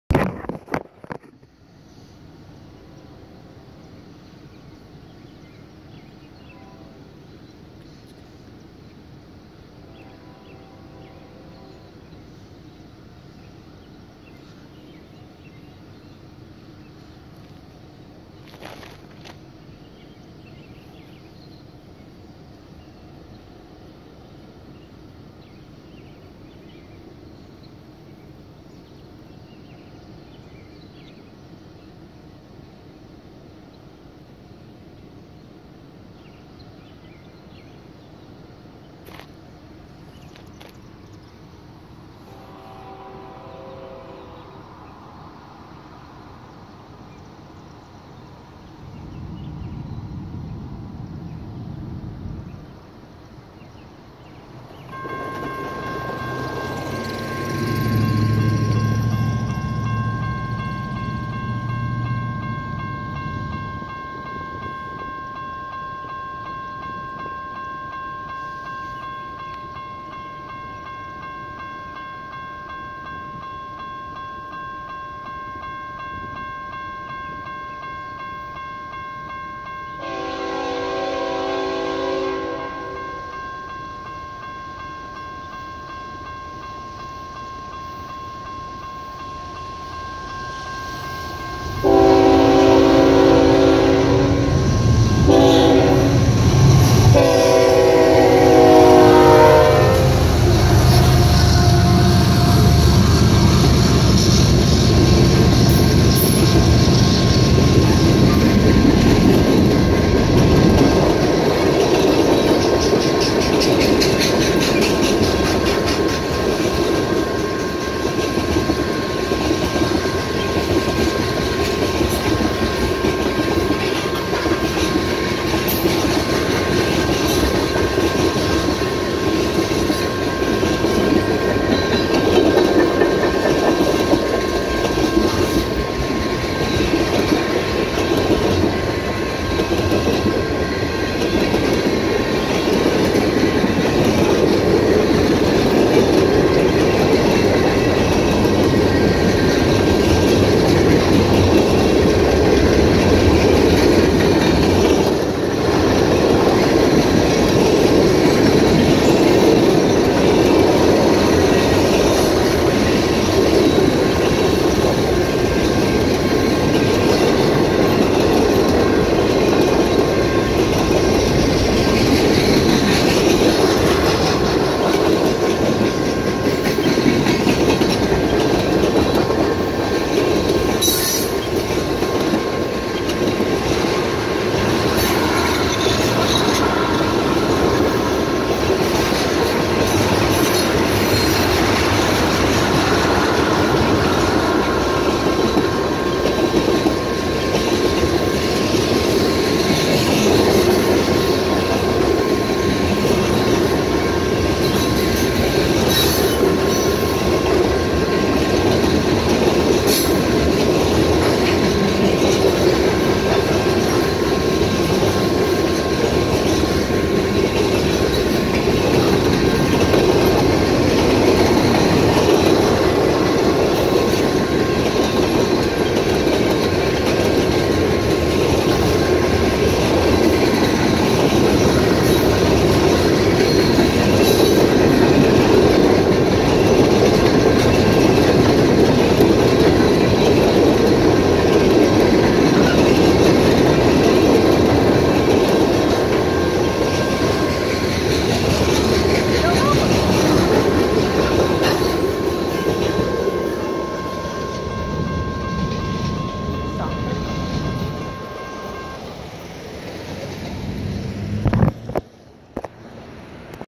CSX Mixed Freight passing through Glenwood Indiana.
Here's a Video I made from a Couple weeks ago Of a CSX Train passing through Glenwood Indiana.